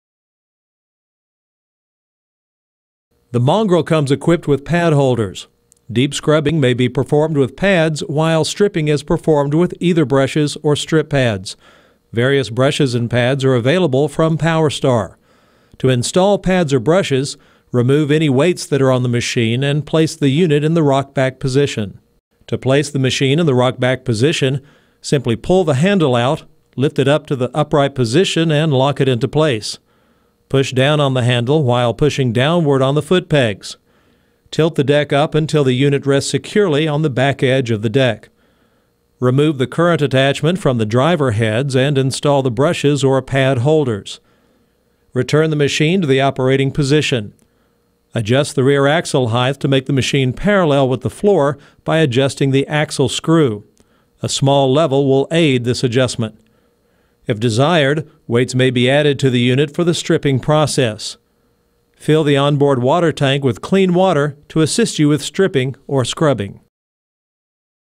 Mongrel Scrubbing
The Mongrel is the first all in one floorcare machine for concrete, stone, and marble surfaces.  One machine, five jobs: Grinder, Polisher, Scraper, Deep Scrubber and Stripping Machine.  Portable: easily fits into a van, pickup or trailer.  Fast, efficient and cost-effective.  Wet or dry grinding.  No cumbersome cords or bulky generator.